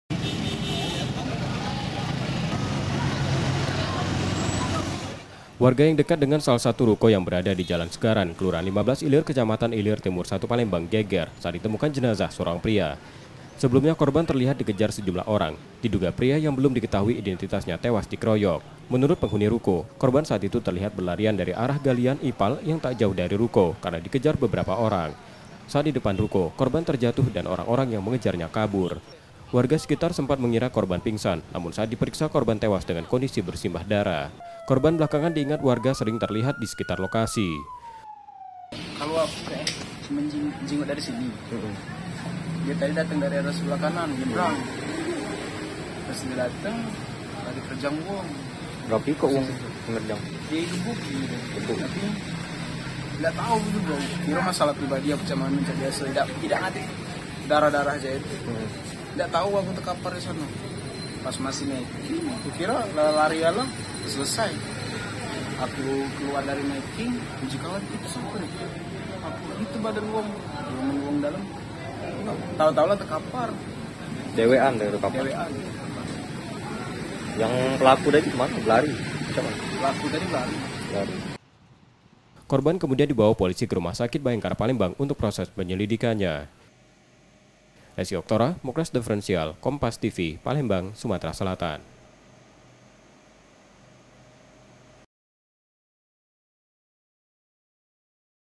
Geger! Seorang Pria di Palembang Tewas Dikeroyok
PALEMBANG, KOMPAS.TV - Seorang pria yang belum dikethaui identitasnya ditemukan tewas di depan sebuah ruko di Palembang. Korban tewas setelah sebelumnnya dikejar beberapa orang.

Warga yang dekat dengan salah satu ruko yang berada di jalan segaran, kelurahan 15 Ilir, kecamatan Ilir Timur I Palembang geger, saat ditemukan jenazah seorang pria.

Sebelunya korban terlihat dikejar sejumlah orang. Diduga pria yang belum diketahui identitasnya tewas dikeroyok.

Menurut penghuni ruko, korban saat itu terlihat berlarian dari arah galian ipal yang tak jauh dari ruko, karena dikejar beberapa orang.

Saat di depan ruko, korban terjatuh dan orang-orang yang mengejarnya kabur.

Warga sekitar sempat mengira korban pingsan, namun saat diperiksa korban tewas dengan kondisi bersimbah darah.

Korban belakangan diingat warga sering terlihat di sekitar lokasi.

Korban kemudian dibawa polisi ke Rumah Sakit Bhayangkara Palembang untuk proses penyelidikannya.